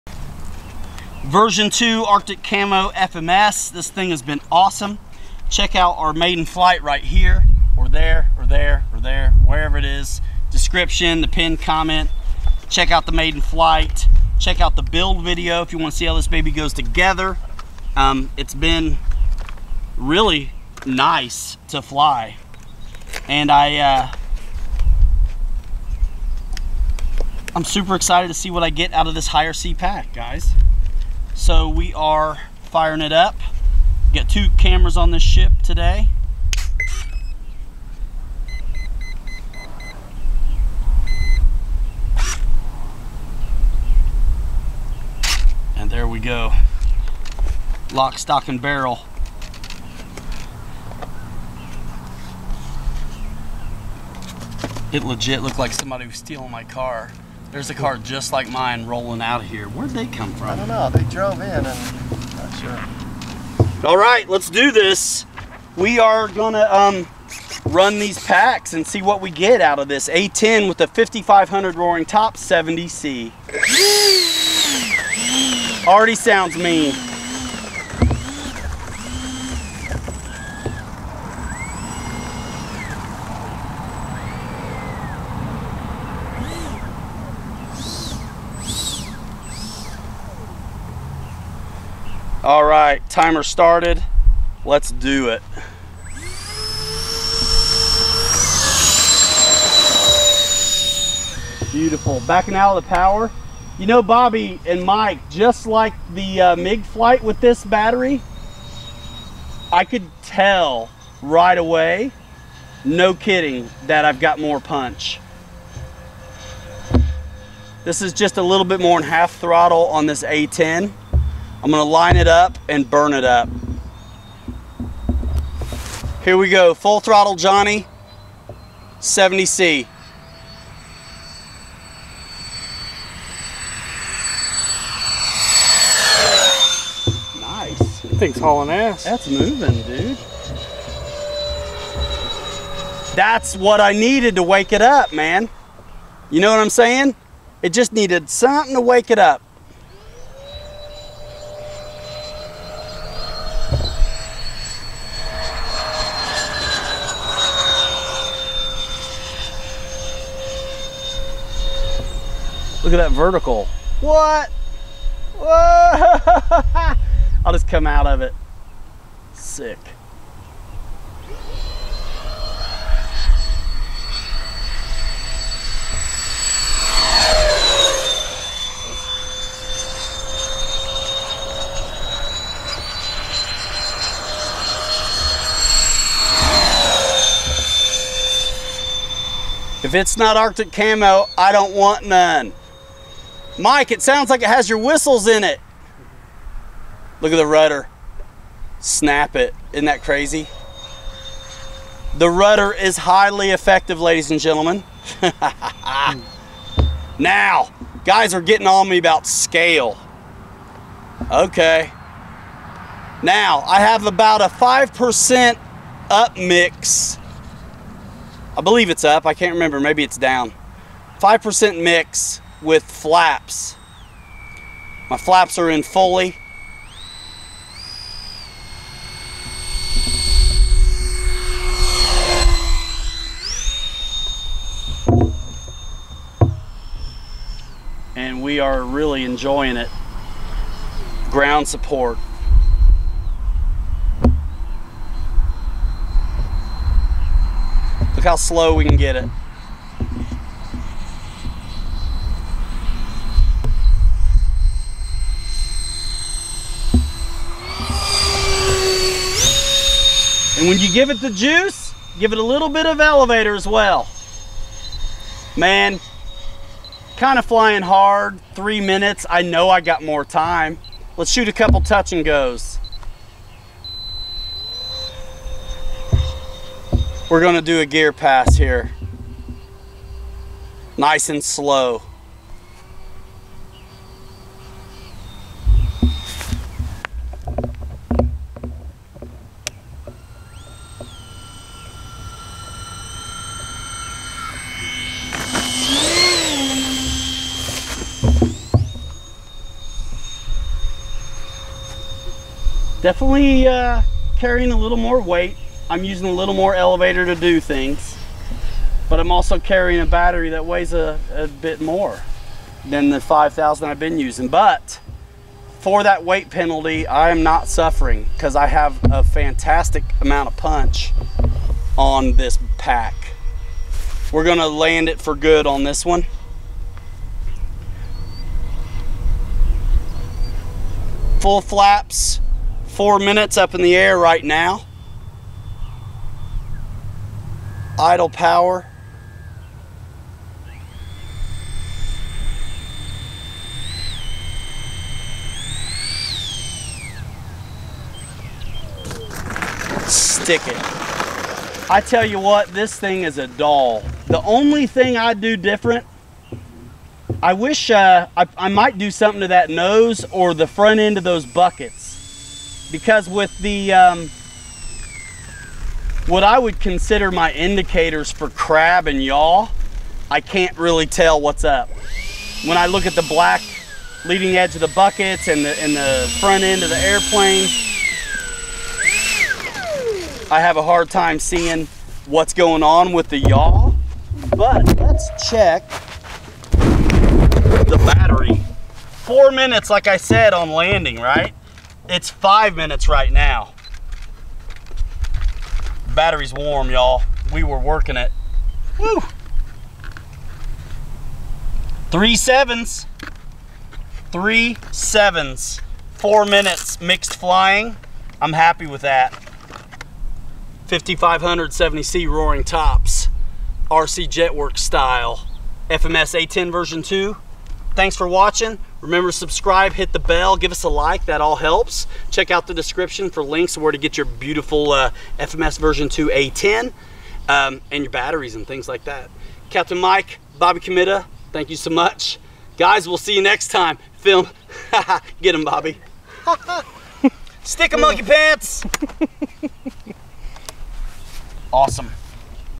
FMS A10 Thunderbolt V2 with RCJETWERX 5500 70c Lipo
The FMS A-10 Twin 70mm EDF V2 Arctic camo is a good
In this video we used one of our RCJETWERX High C rated 5500 70c 6s Roaring Top Lithium Polymer Batteries.